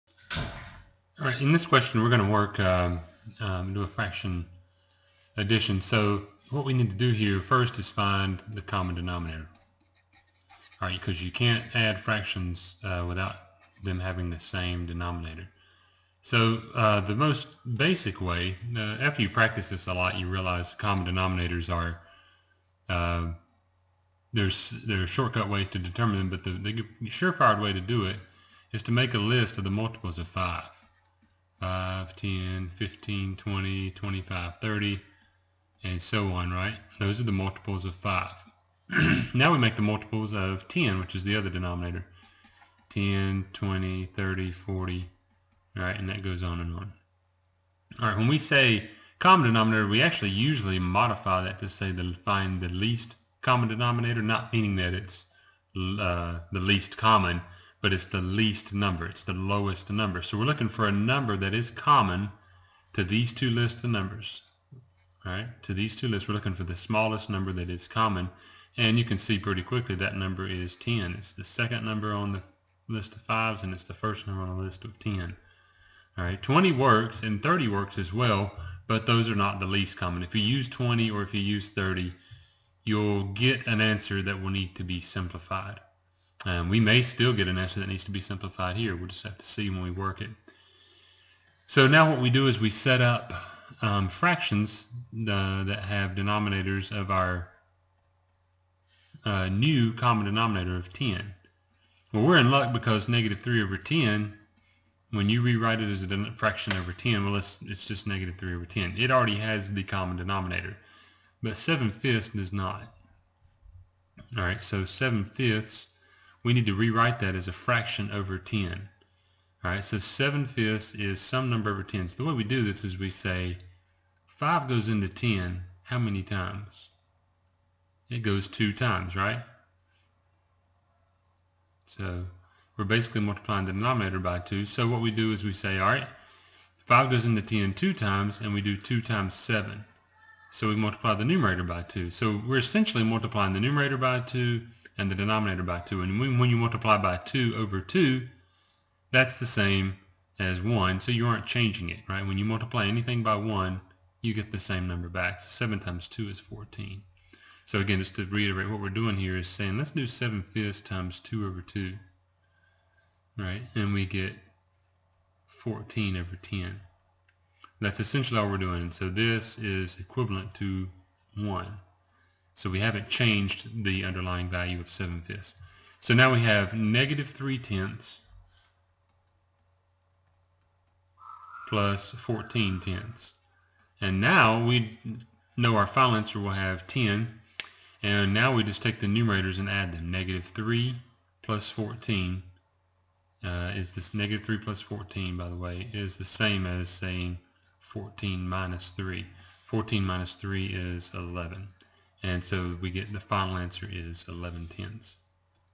Real numbers - adding fractions
Adding positive and negative fractions

MyMathLab, Bittinger/Beecher, Developmental Mathematics, 8th edition. Section 7.3, #39